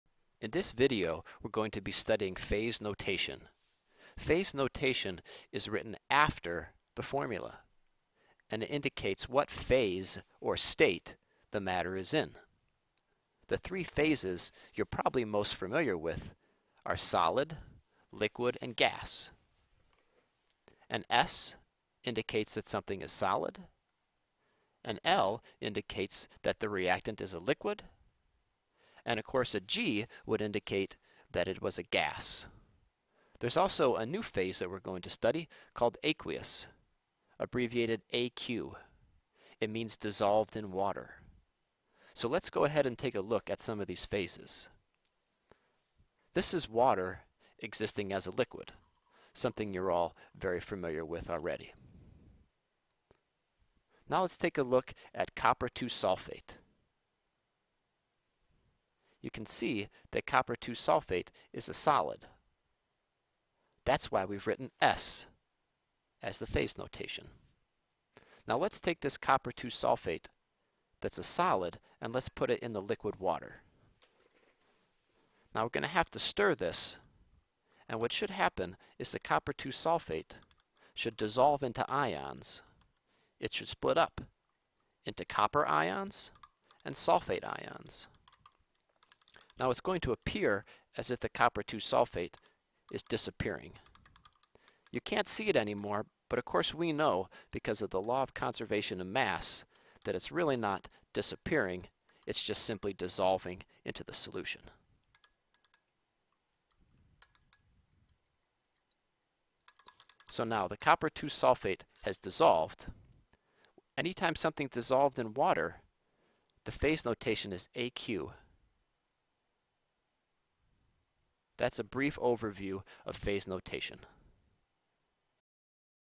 Phase Notation
This video shows how to write chemical phase notation